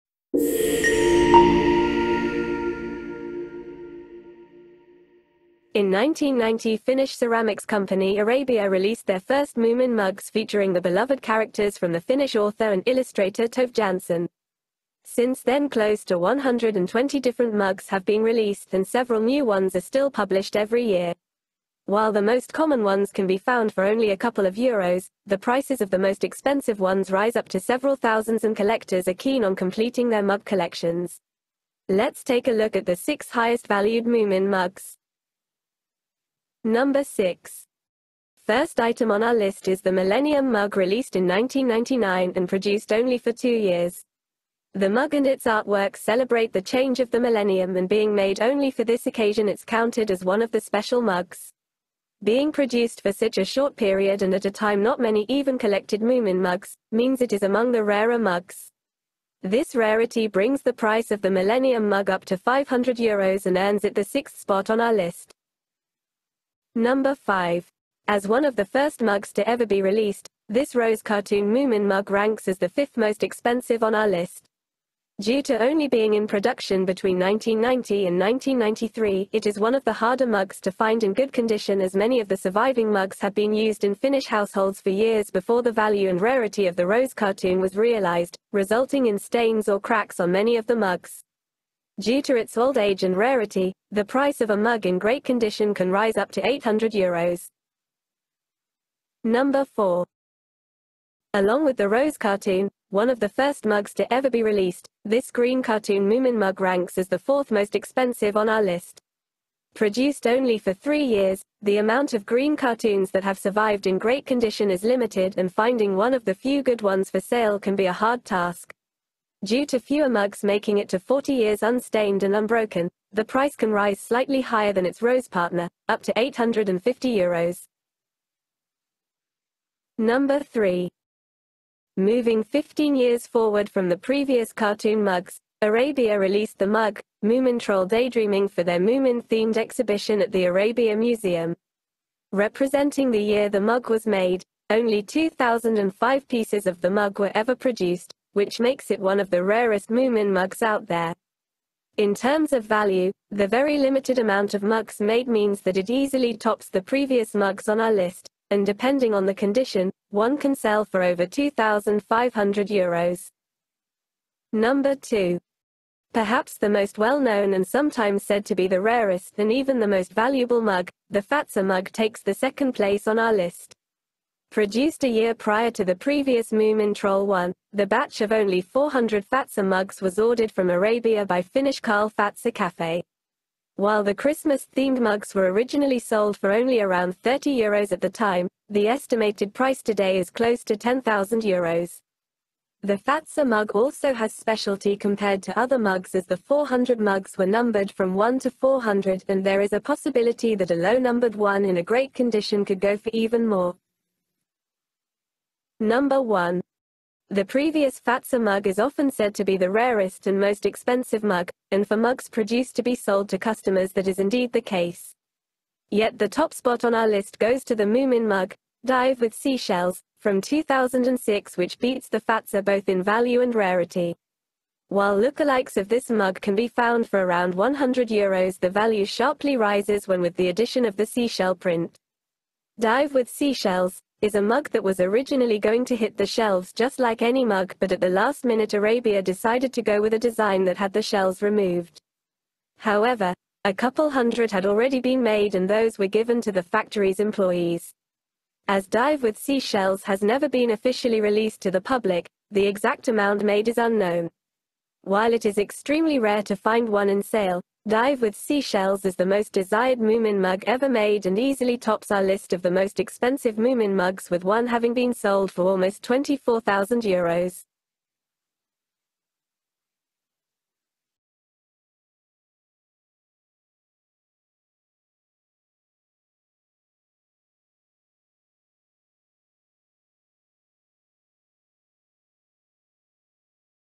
TOP 6 Most Expensive Finnish Moomin Mugs | Design & Collecting
The most expensive Finnish Moomin mugs collectors are trying to get their hands on.